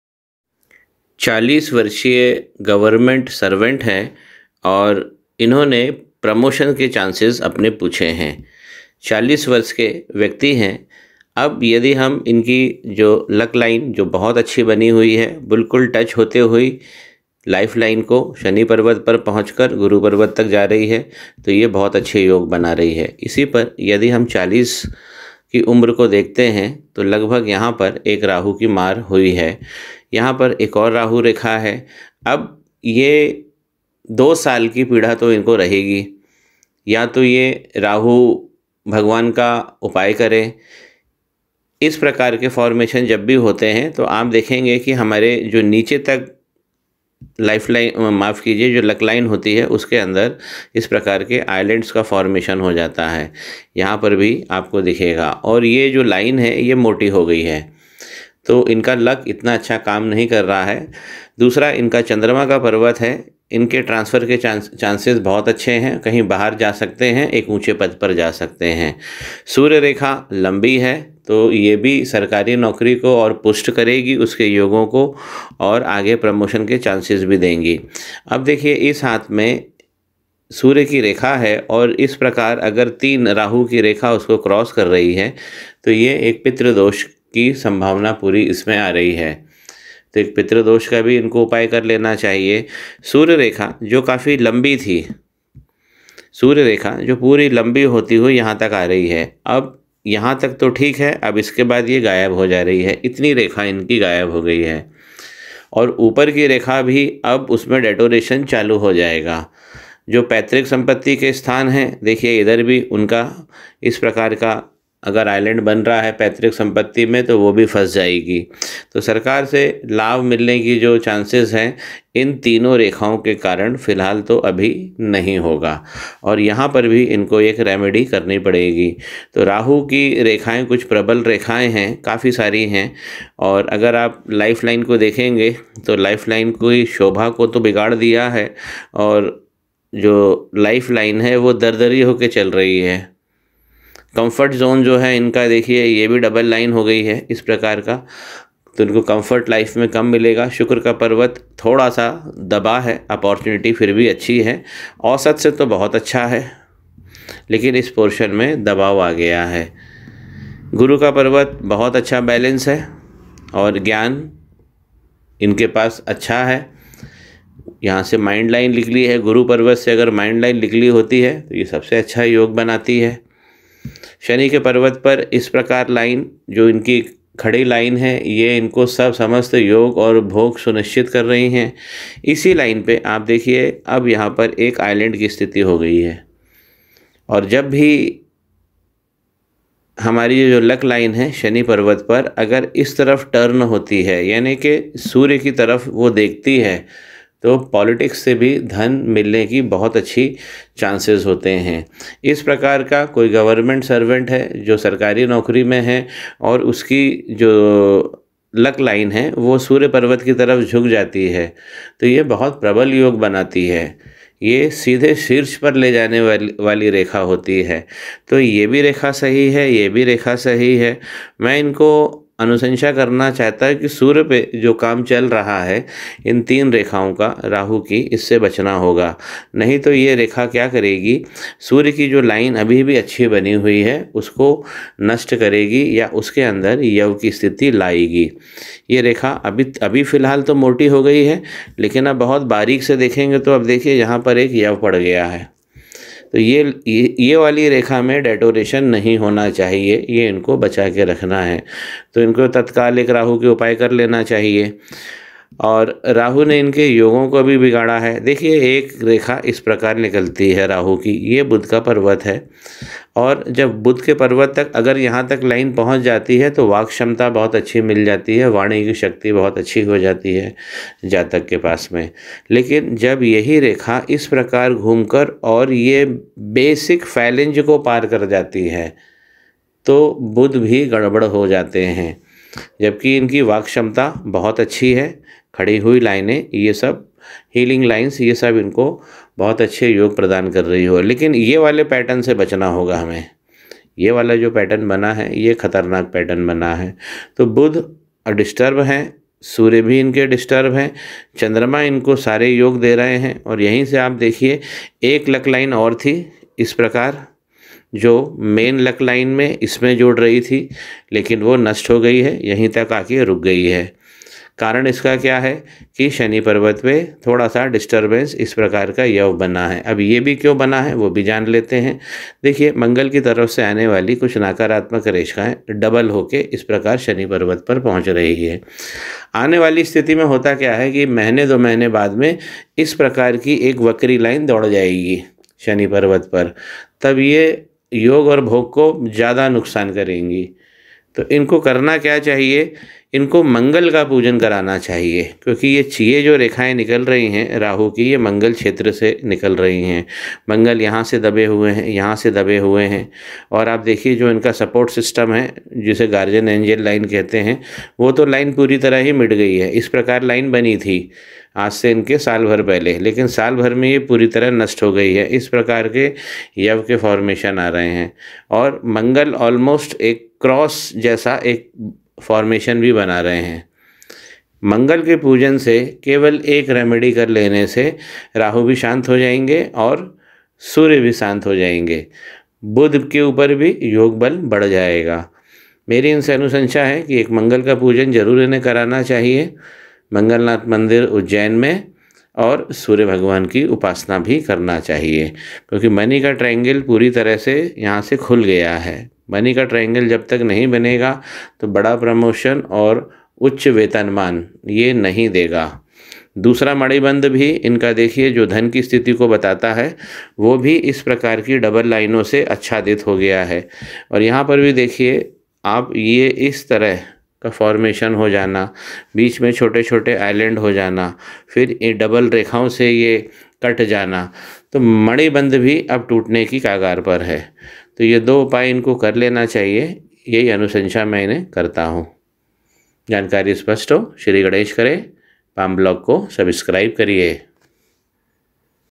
🌞Govt.Officer must Protect Sun power/सूर्य पर्वत का ध्यान रखिए#Palm Blog#Astrology
Promotion/Higher Increment यहीं से मिलता है/पितृ दोषों से बचकर